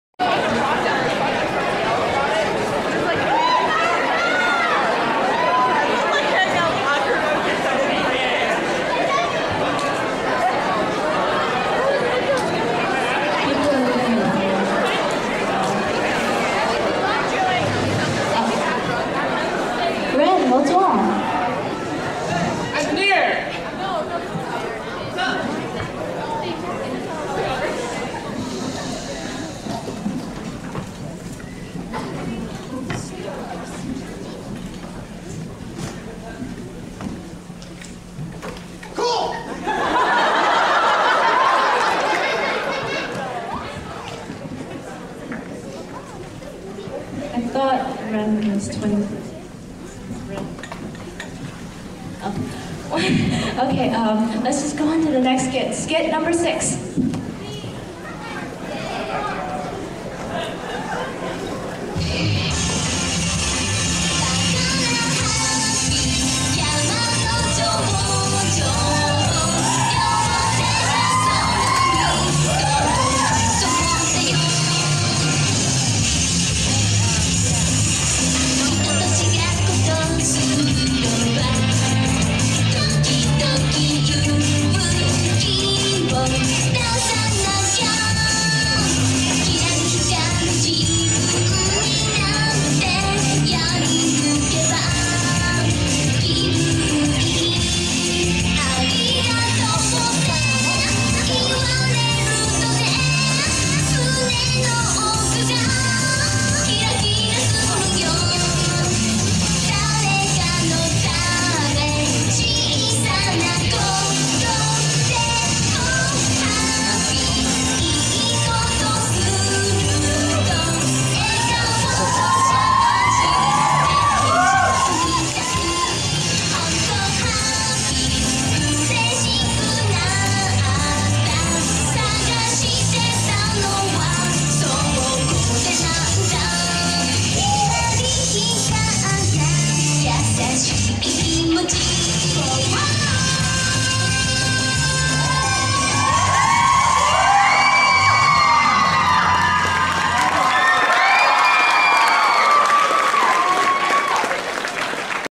Sac Anime Summer 2008 - #06 KR
Video of Entry 06 , cosplay skit of Kirarin Revolution performed at Sac Anime Summer 2008.  (Permission granted)